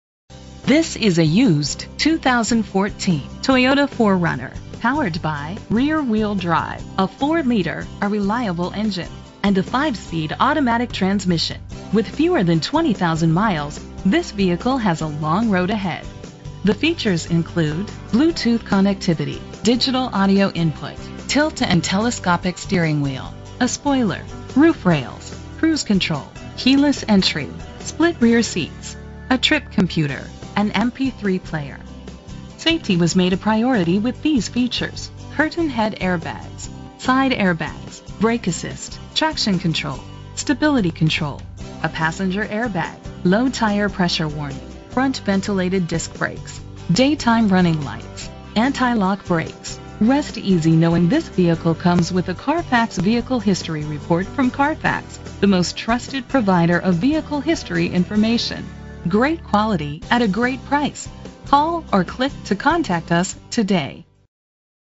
2014 Toyota 4Runner - North Augusta SC
Year: 2014
Make: Toyota
Model: 4Runner
Trim:
Engine: 4.0 liter 6 cylinder 24 valve MPFI DOHC
Transmission: 5-Speed Automatic
Color: Silver
Mileage: 18668
Address:
5590 Jefferson Davis Hwy